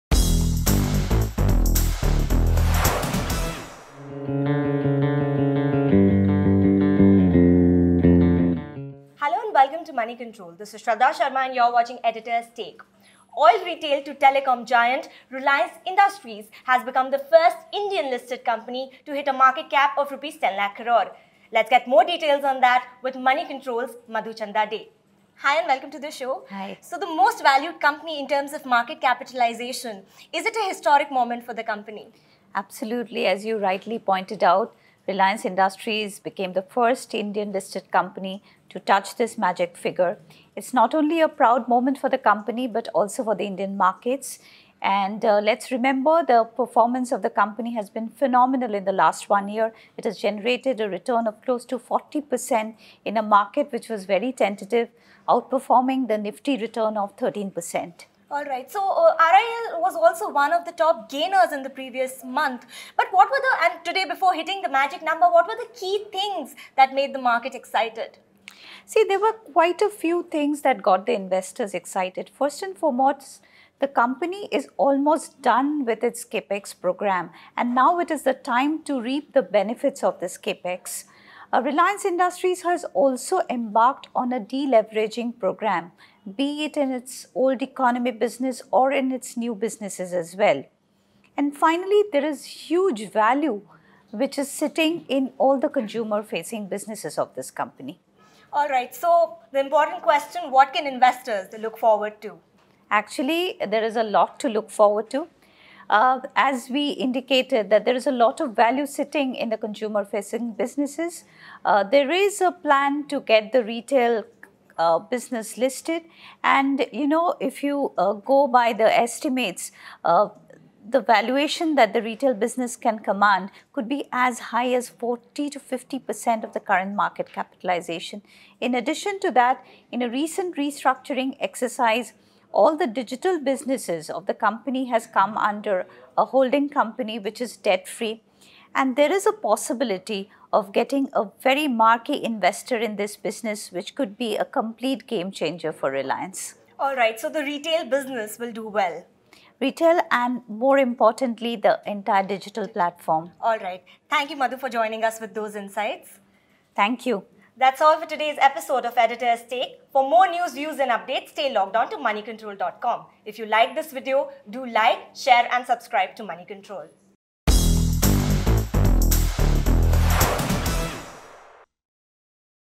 Editor's Take | RIL: First Indian Company To Reach Market Capital Of 10 Lakh Crore
Reliance Industries has become the first Indian listed company to hit a market capital of 10 lakh crore rupees. Shraddha Sharma is in conversation with Madhuchanda Dey to discuss the same.